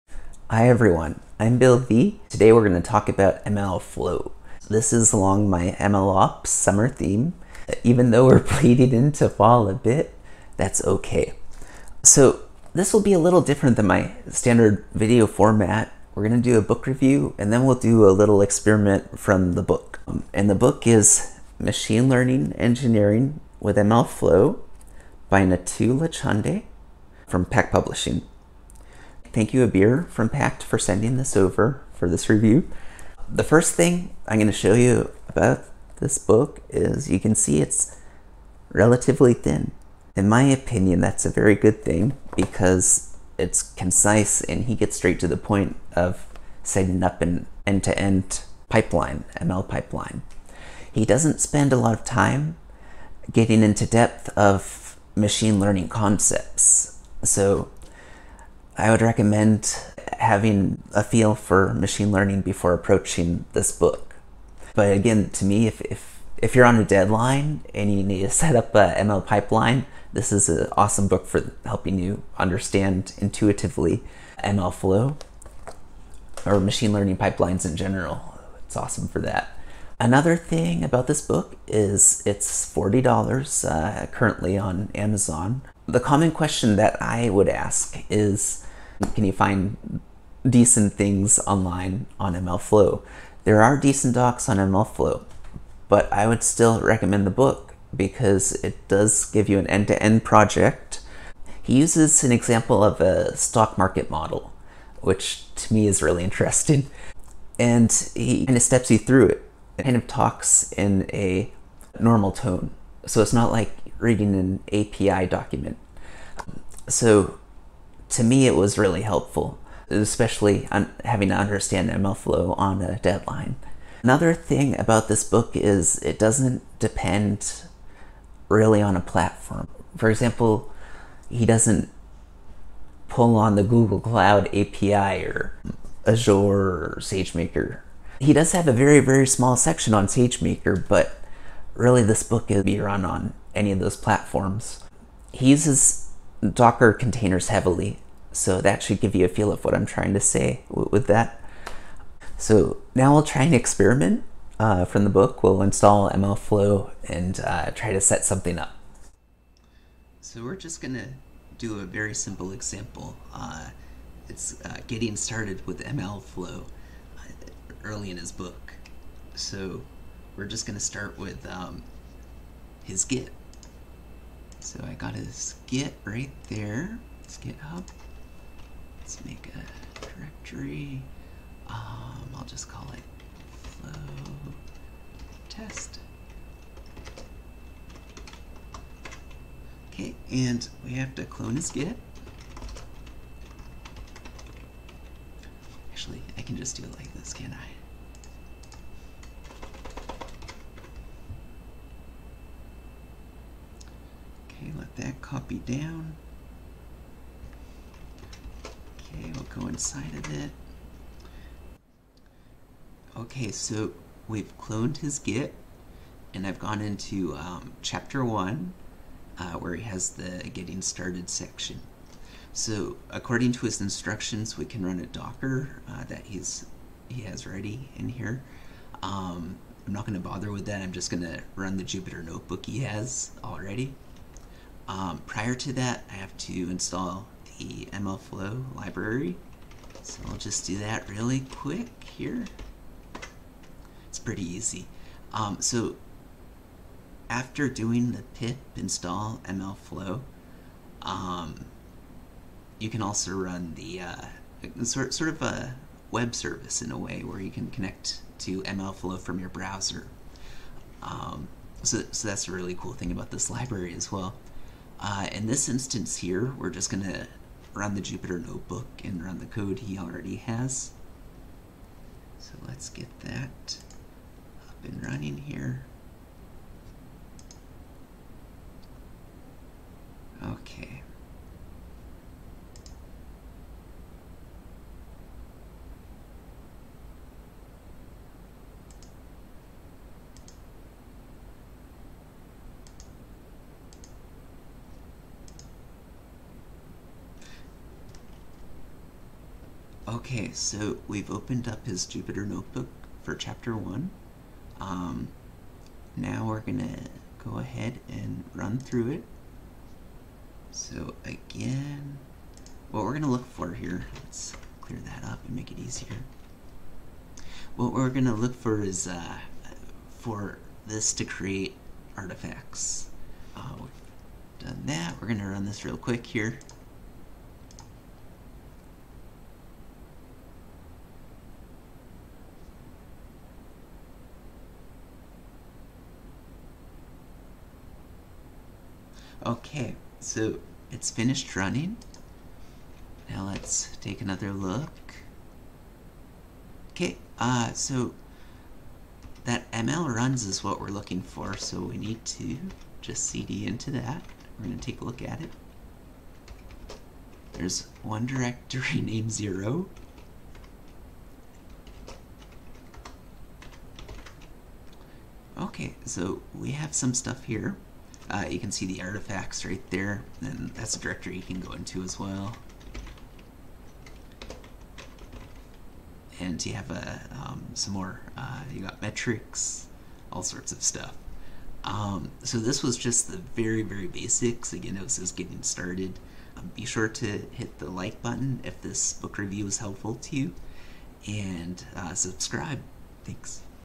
Machine Learning Engineering with MLflow
A book review, Machine Learning Engineering with MLflow, by Natu Lauchande from Packt Publishing.